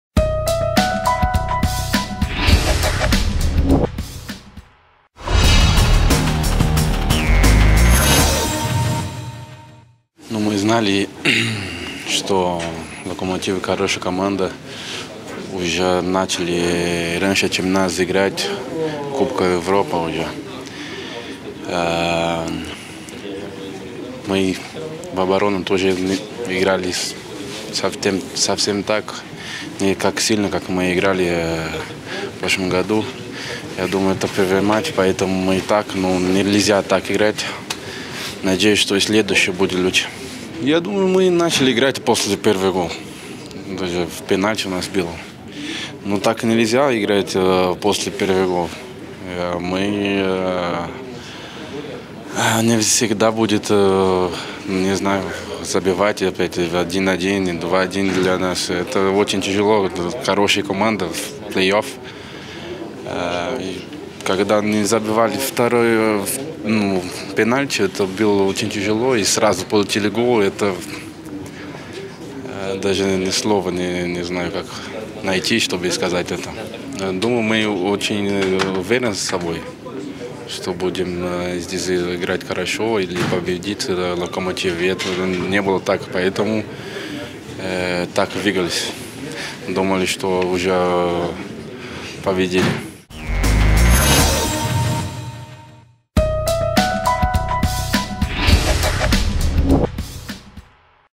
После матча: Зелао
Интервью защитника ФК "Кубань" Зелао (Луис Рикардо Да Силва) после матча с московским "Локомотивом"

Стадион "Локомотив", Москва, 3 марта 2012